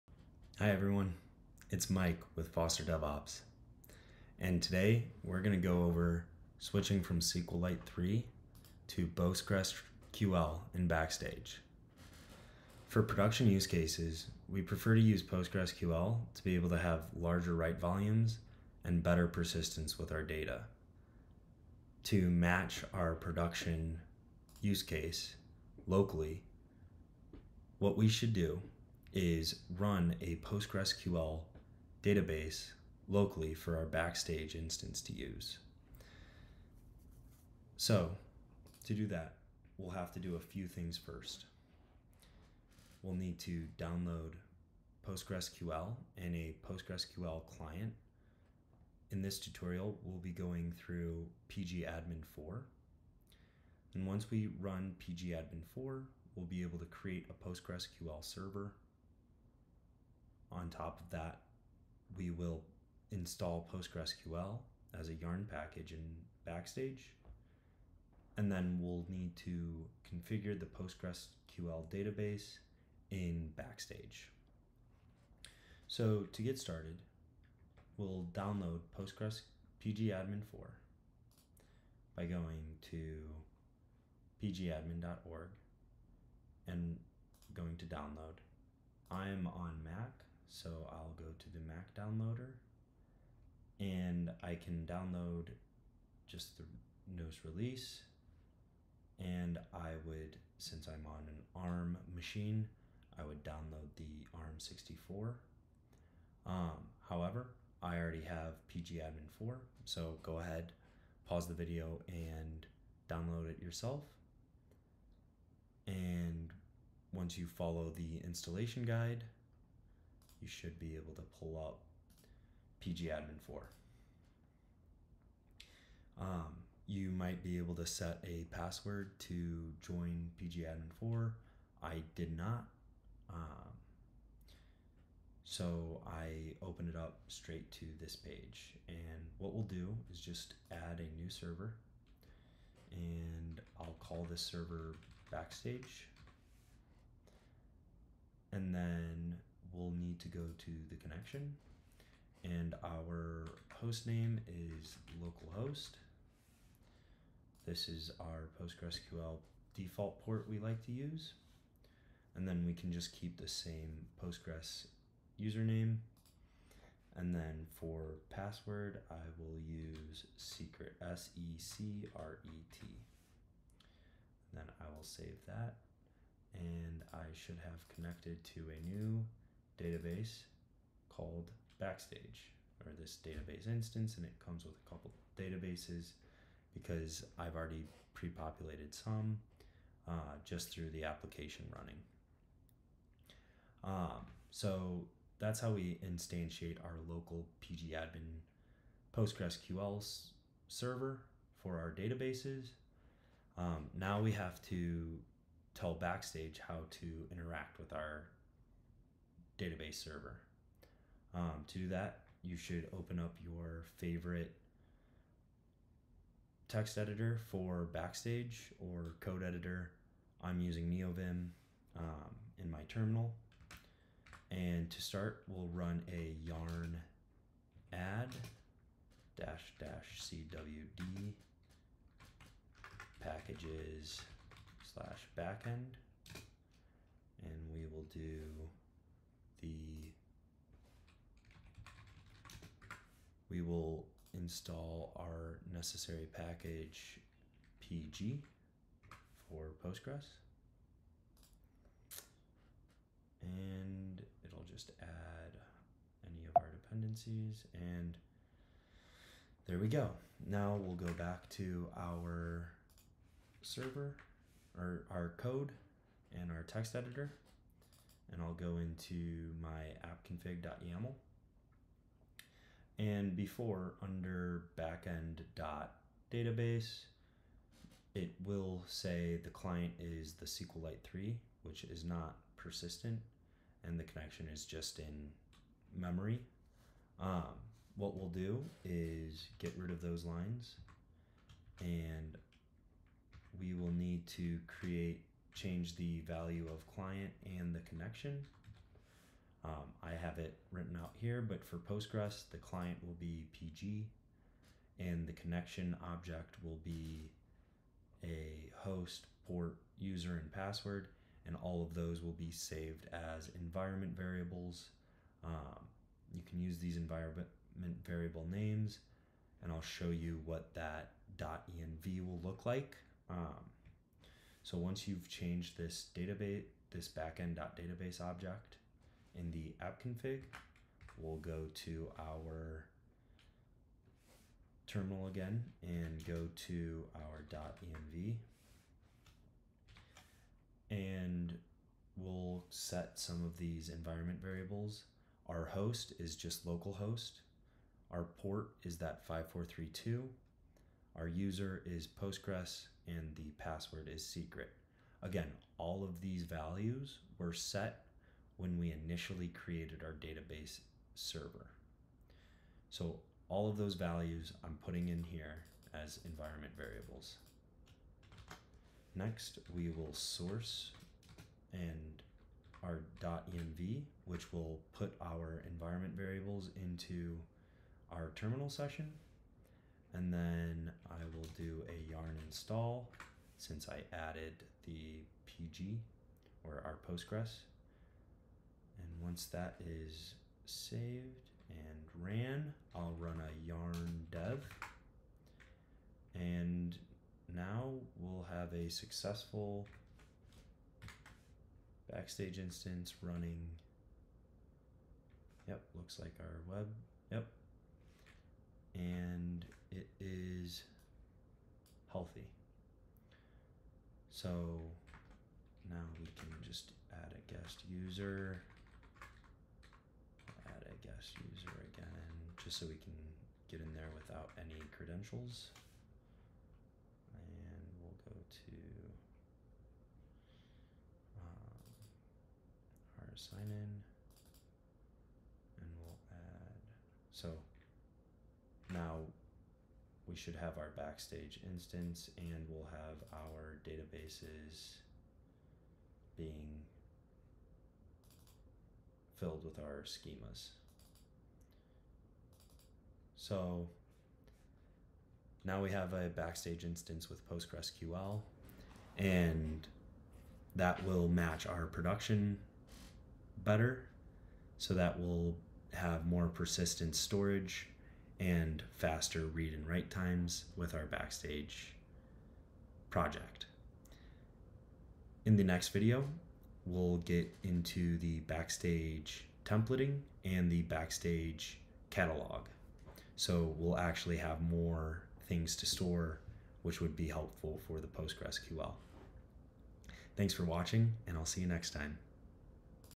How to use PostgreSQL in a local Backstage Application
In this episode, we set up Postgresql locally using PGAdmin4. We then make the necessary changes in our Backstage application to run locally.